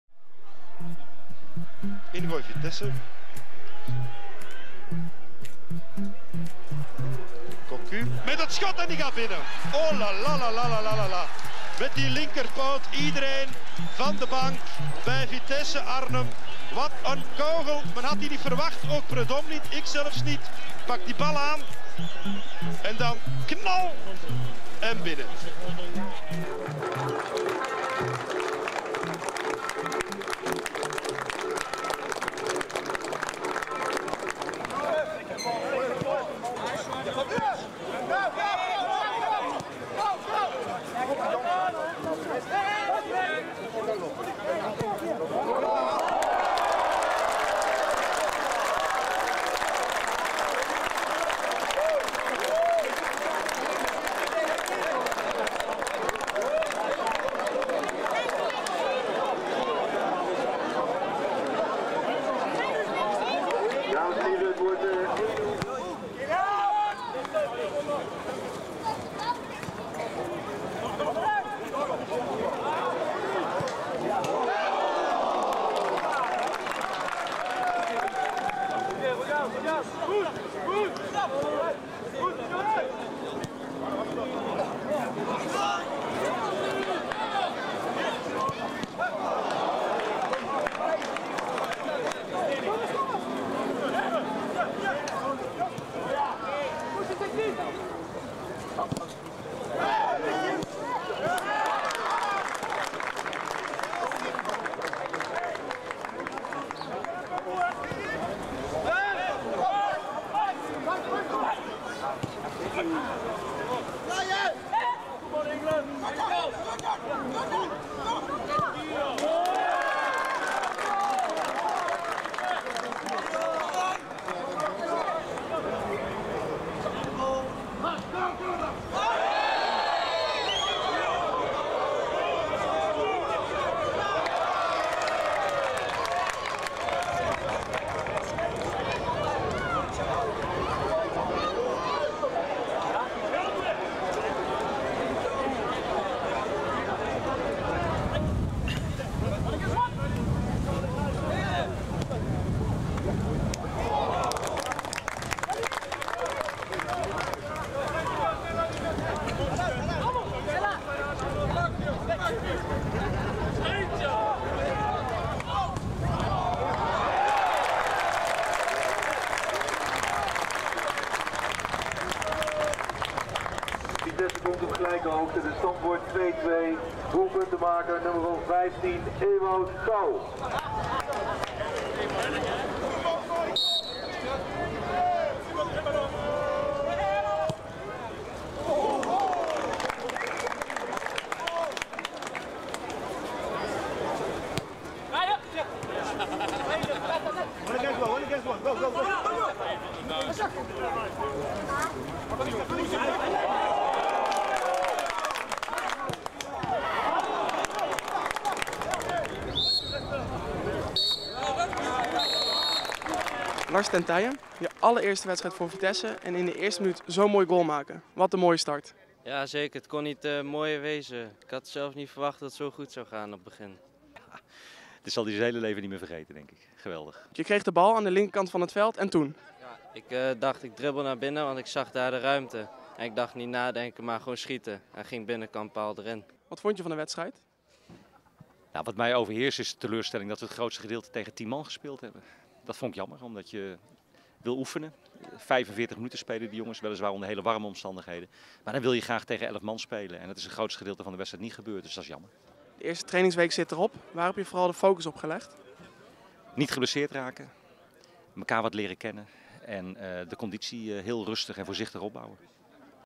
Samenvatting Vitesse vs KV Oostende
Vitesse heeft de eerste oefenwedstrijd van de voorbereiding 2015|2016 met 2-2 gelijkgespeeld tegen KV Oostende. Jeugdspelers Ten Teije en Gouw waren trefzeker. Hoofdtrainer Peter Bosz liet al zijn selectiespelers 45 minuten opdraven op het zeer warme sportpark van ESA.